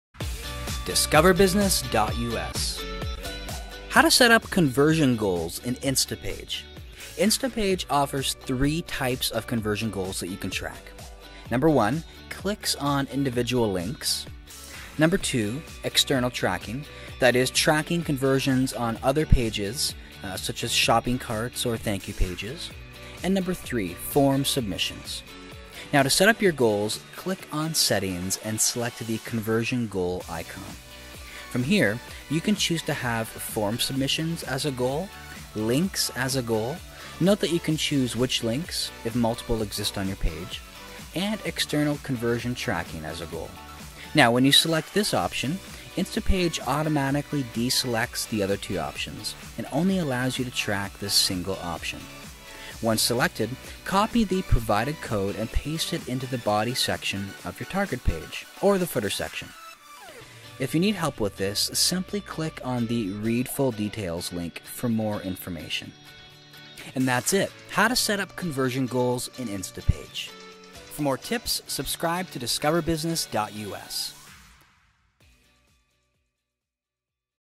How to Setup Conversion Goals | Instapage Landing Page Builder
Learn how to set-up conversion goals in Instapage. By doing this, you’ll be able to track and analyze the exact actions you want.

Discoverbusiness.us

How to Setup conversion goals in Instapage

Instapage offers three types of conversion goals that you can track:  1. clicks on  individual links, 2. external tracking ( shopping carts, thank you pages), and 3. form submissions.

To set your goals up, click on settings and select the Conversion Goals icon.

From here, you choose to have Form submissions as a goal, links as a goal ( note that you can choose which links, if multiple exist on your page), and external conversion tracking.

When you select this option, Instapage automatically deselects the other 2 options, and only allows you to track this single option.

Once selected, copy the provided code and paste it into the body section of your target page ( it’s best to paste it right before the closing body tag.

If you need help with this, simply click on the “Read full details” link for more information.

instapage
tyson quick
Preditive Reach
BizzeBee
Real Econ